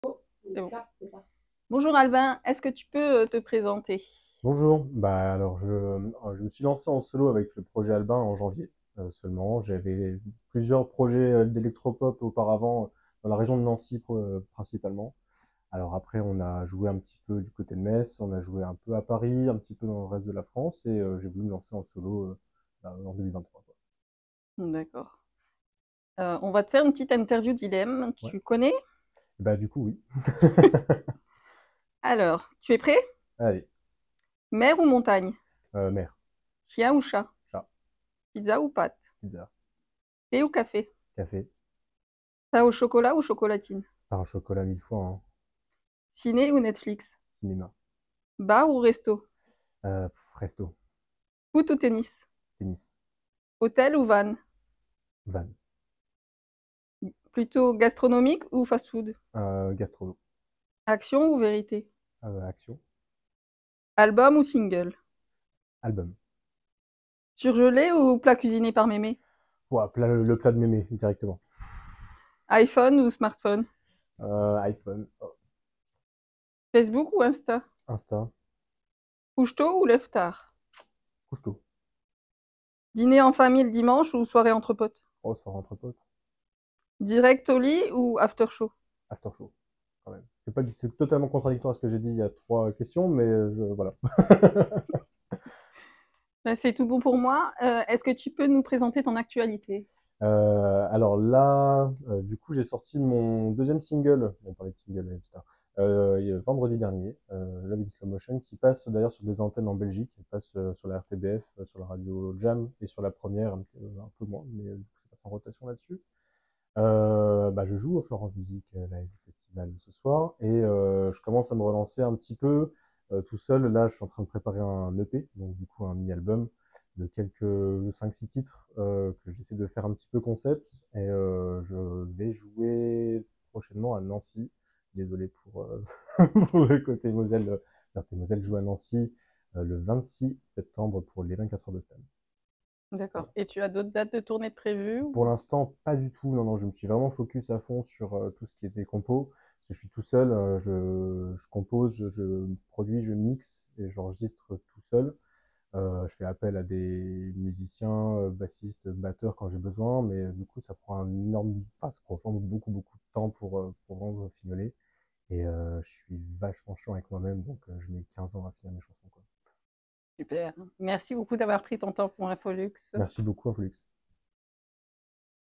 Interview de ALBIN au FLORANGE MUSIC FESTIVAL 2023
Interview de ALBIN au FLORANGE MUSIC FESTIVAL 2023 par notre journaliste Stéphanie le 08/09/2023
Vidéo : 2 La X Photographie